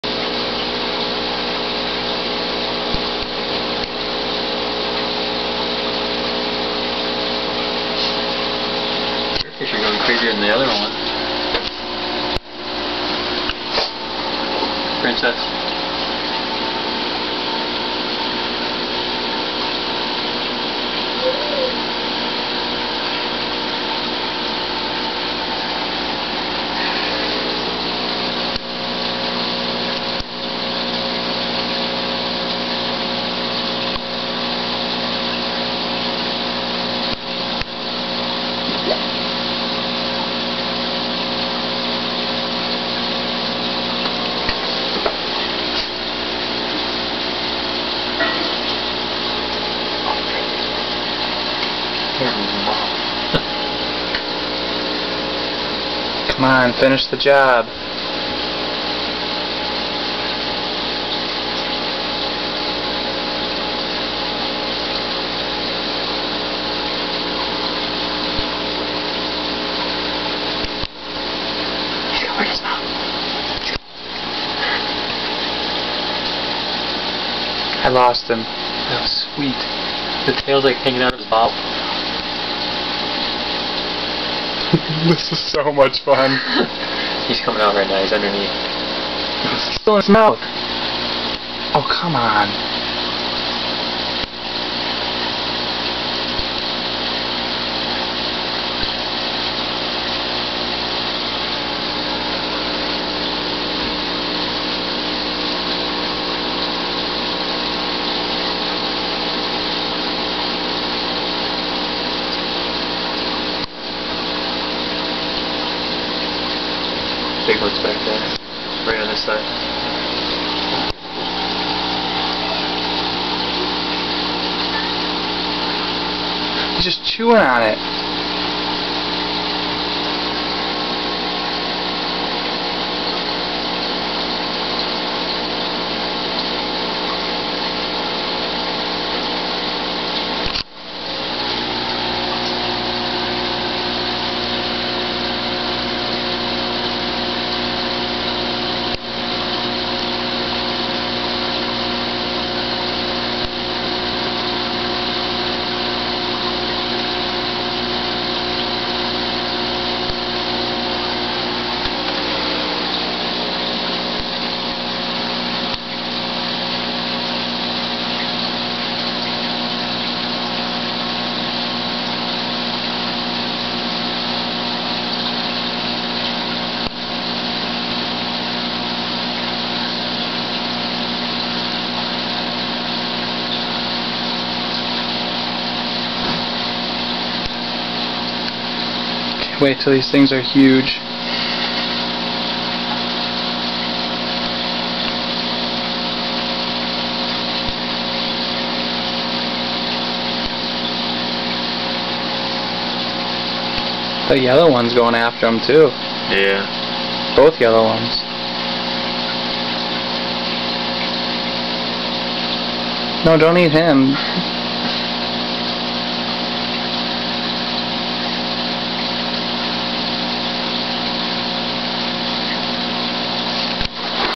Cichlids Eating Feeder Fish
African cichlids eating 11 feeder fish